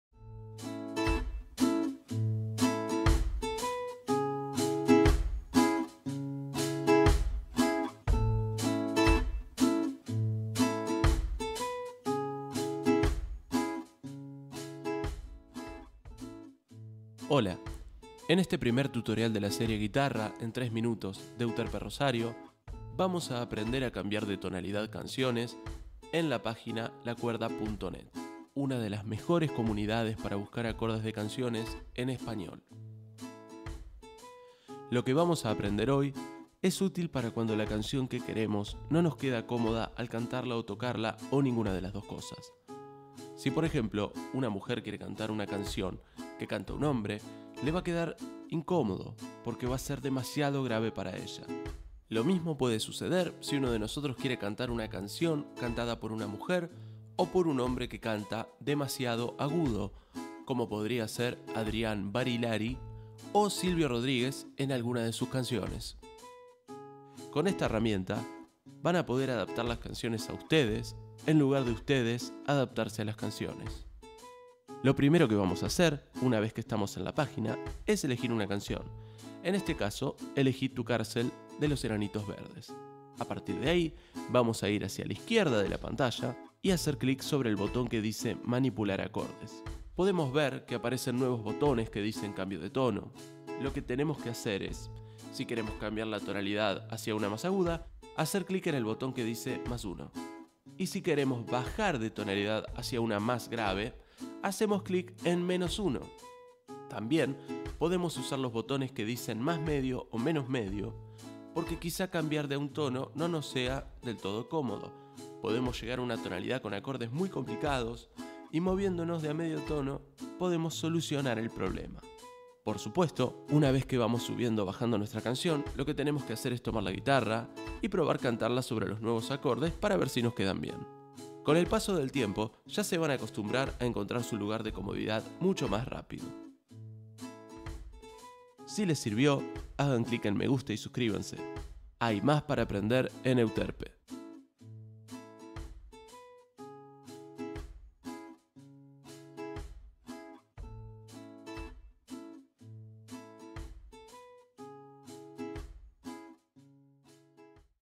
Tutorial - Como cambiar de tono una cancion para tocarla en guitarra
Serie "Guitarra en tres minutos" es una iniciativa de Euterpe Rosario para acercar el conocimiento musical a quien quiera encontrarlo.